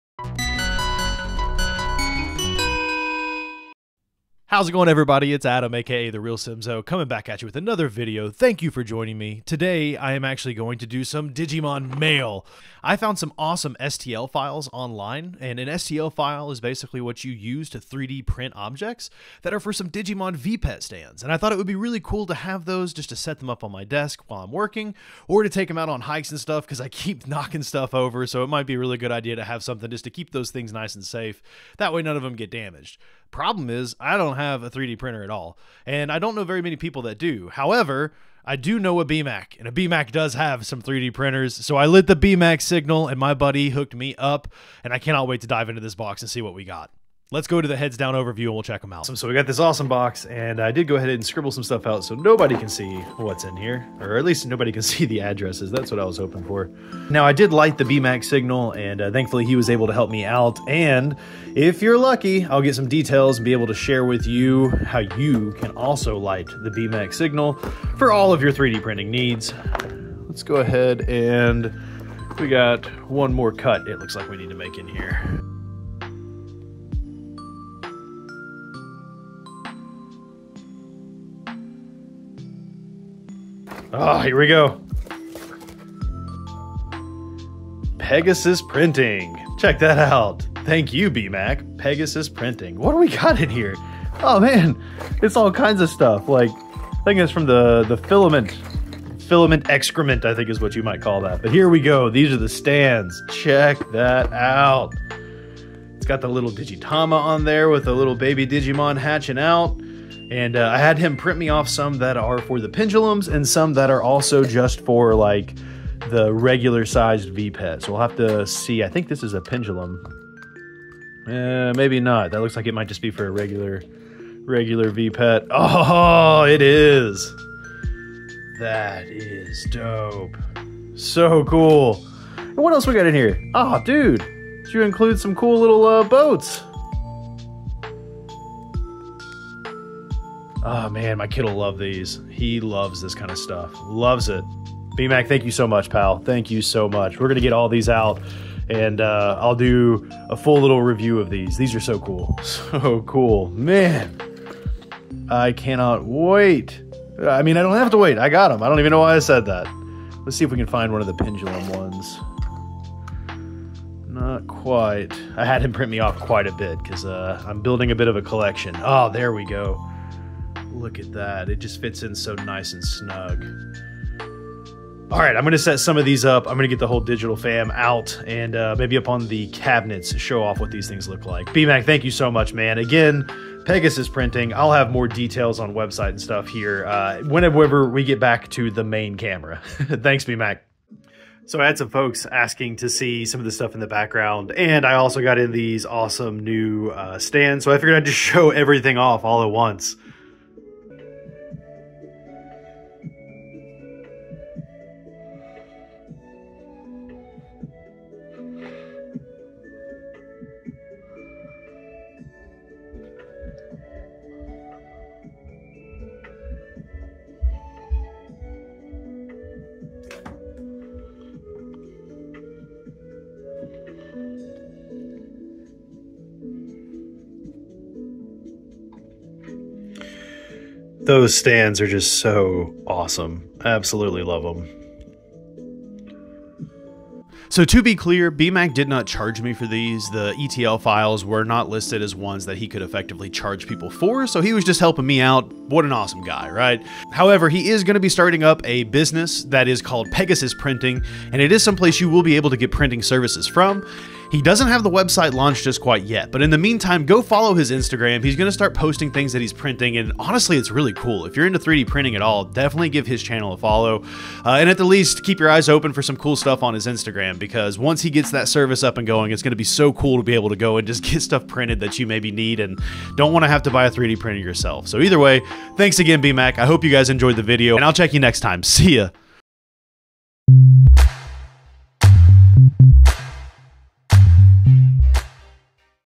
Unboxing 3D-Printed Digimon Stands!
I had my friend BMAC print off some awesome Digimon V-Pet stands. I got a few for the regular brick style and the pendulums! I love how these look and give me the perfect view of the Digmon! I think they look awesome up on the shelf, and it's nice to have them at an angle where I can see them easily through the day! Huge shout out to Takoza on Printables for designing these:

Another huge shoutout to Pegasus Printing for hooking me up! Be sure to follow them on Instagram

👍 Like if you enjoyed this episode!